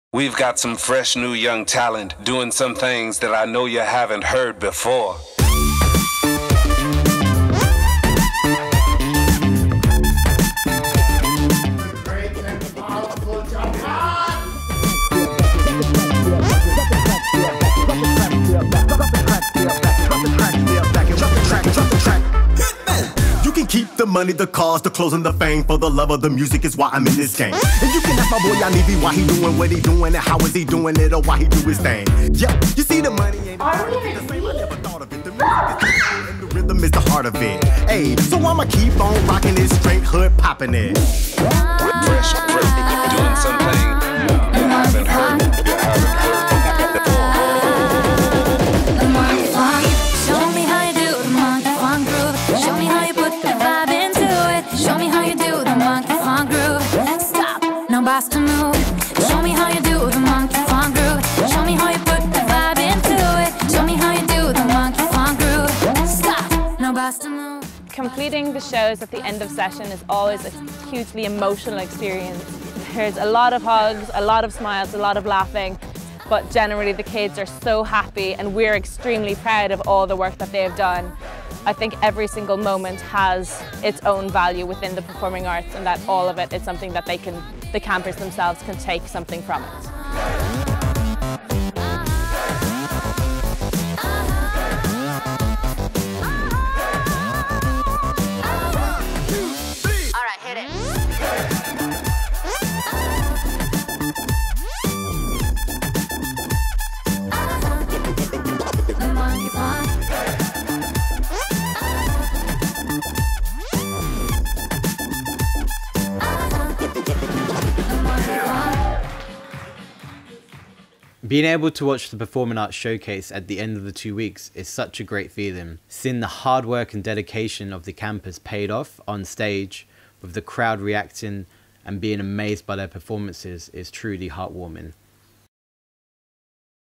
Performing Arts - Village Camps Style!
A little segment on the Village Camps' Performing Arts Camp in York, England.